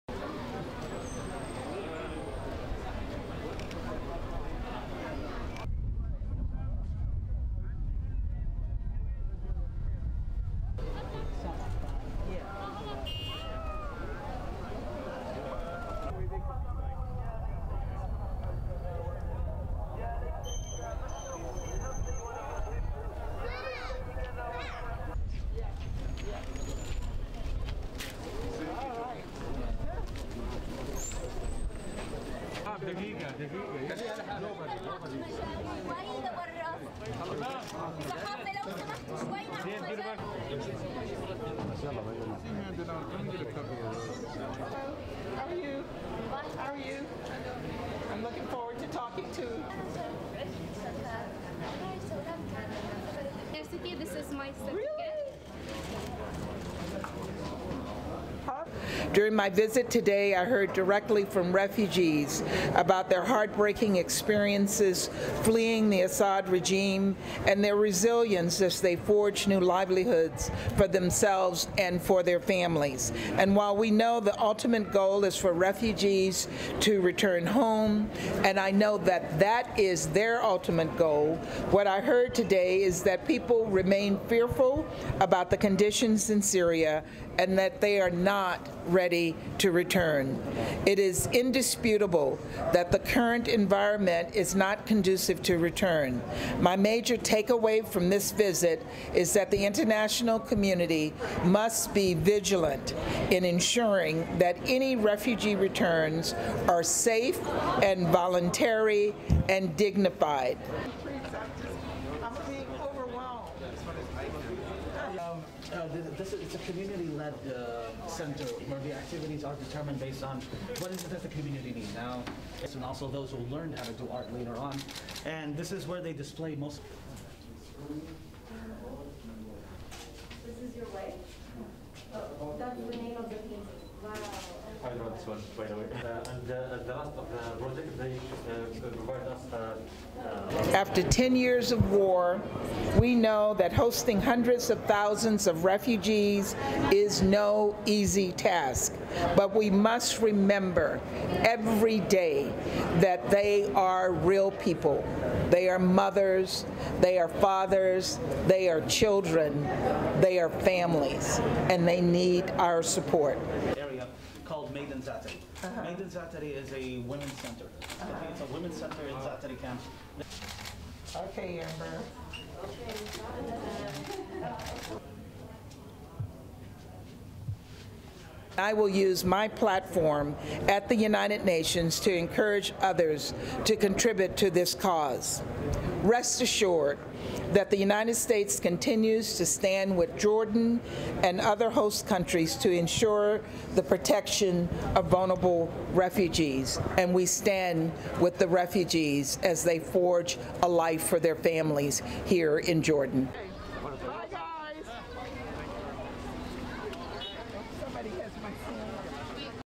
US ambassador to UN: Syrians afraid to return home
(19 Nov 2021) LEAD IN:
After speaking with Syrian refugees in Jordan, the United States representative to the United Nations, said many of them are "fearful" of the conditions in Syria.
The international community must ensure that "refugee returns are safe," Linda Thomas-Greenfield said during her visit.

STORY-LINE:
The ultimate goal of Syrian refugees in the Zaatari Camp is to return home but the "current environment is not conducive to return," the United States representative to the United Nations said on Friday.
Linda Thomas-Greenfield's remarks came after she spoke with Syrian refugees at the camp during her visit to Jordan.
"My major takeaway from this visit is that the international community must be vigilant in ensuring that any refugee returns are safe and voluntary and dignified," she said.
More than 5.5 million Syrians have fled the country since the war broke out in 2011, with most settling in neighboring countries like Jordan.
The Zaatari refugee camp alone hosts approximately 80,000 refugees and Jordan hosts the second largest number of refugees per capita in the world.
"After 10 years of war, we know that hosting hundreds of thousands of refugees is no easy task, but we must remember every day that they are real people," Thomas-Greenfield said.
Jordan hosts a total of more than 650,000 Syrian refugees, most of whom live outside of camps.
"Rest assured that the United States continues to stand with Jordan and other host countries to ensure the protection of vulnerable refugees," Thomas-Greenfield said.
The United States is among the top donors supporting the refugee response in Jordan.